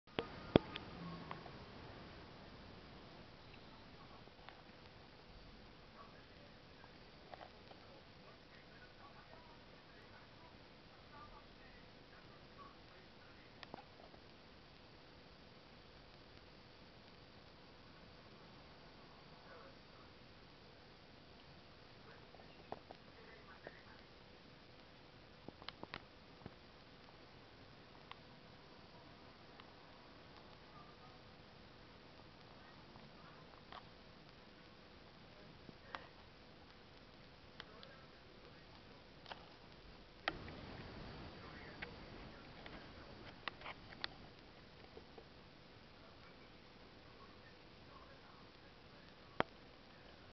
Ježek bělobřichý - Moje Jůlinka
Julča šmejdí a hrabe se pod deku :-))